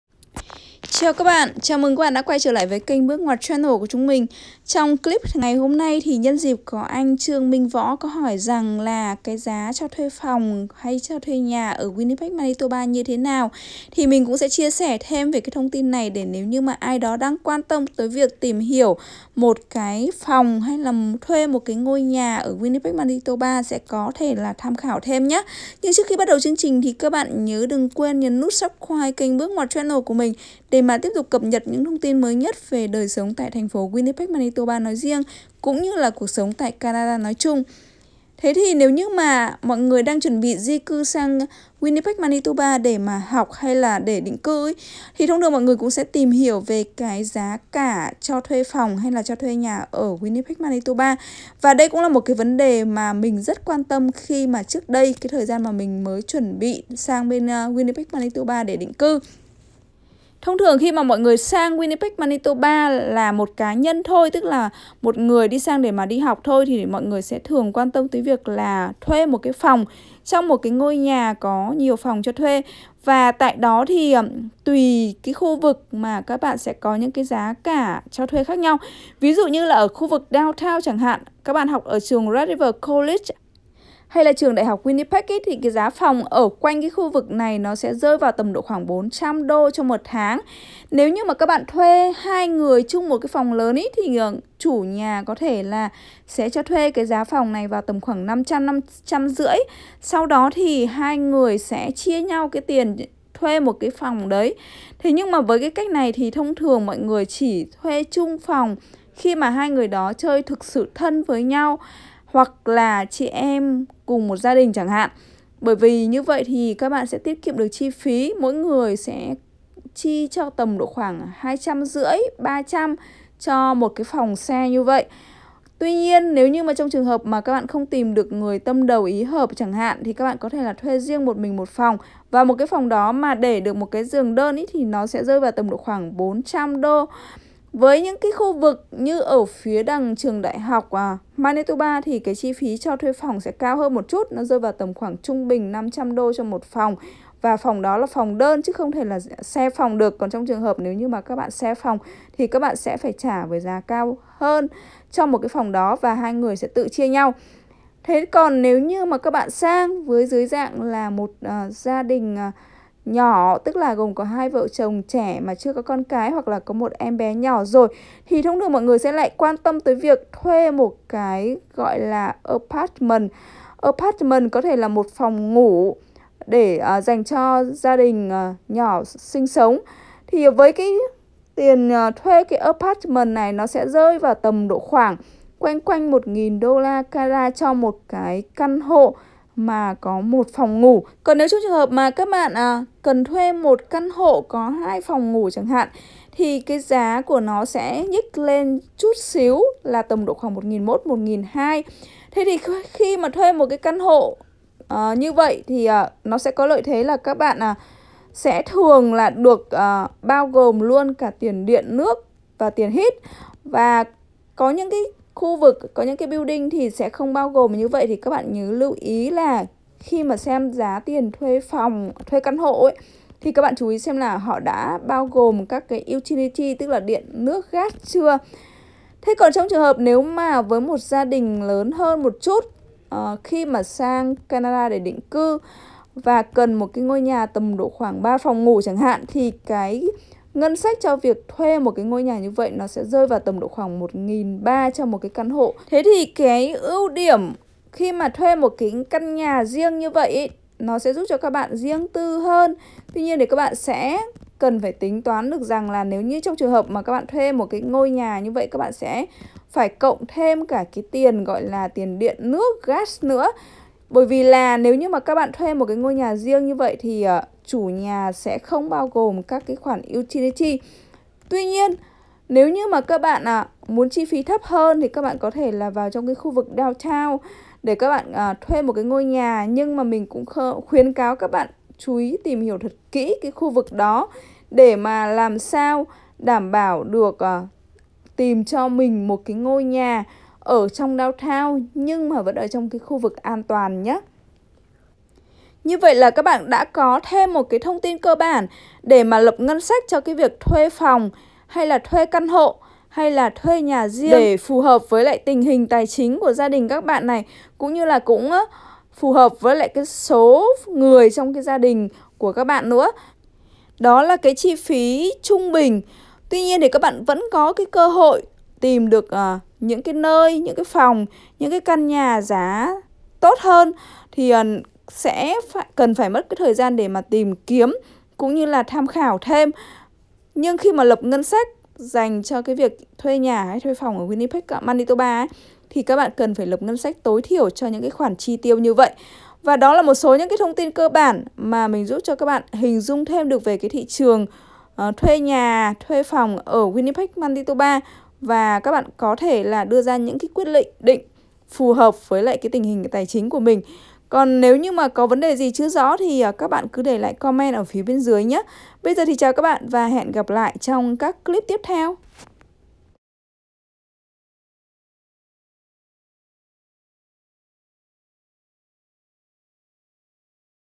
Giá cho thuê phòng hay cho thuê nhà ở Winnipeg, Manitoba như thế nào?
Giá nhà hay giá phòng cho thuê ở Winnipeg so với mặt bằng chung các thành phố khác là khá hợp lý. Tuỳ thuộc vào tình hình tài chính của các bạn mà sẽ có các dạng phòng hay nhà cho thuê phù hợp.